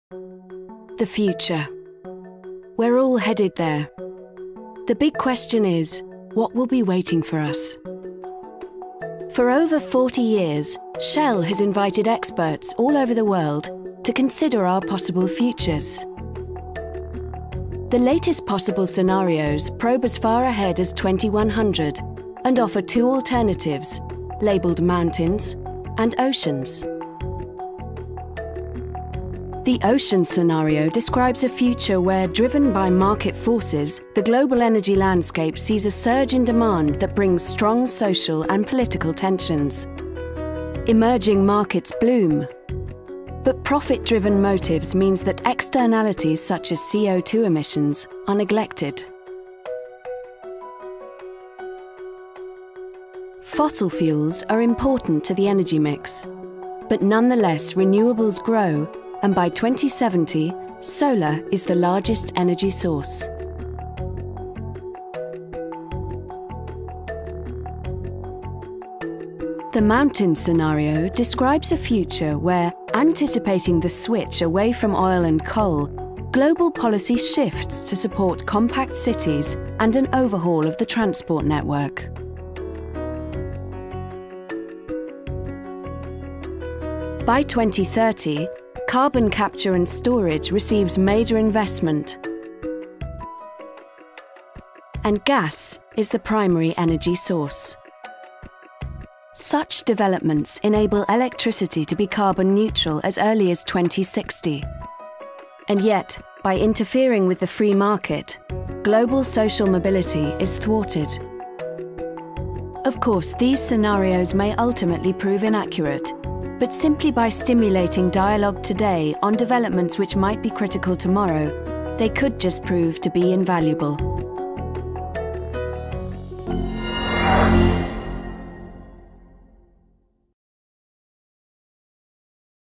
Mountain and Oceans Scenarios
THE FUTURE IS IN OUR HANDS
For over four decades, Shell has reached out to experts all over the world to develop scenarios that consider future. Such investigations seek to discover how political trends, environmental drivers, economic cycles, technological uncertainties and social dynamics might shape the global energy system. If nothing else, Shell Scenarios stimulate dialogue on developments which may prove critical one day.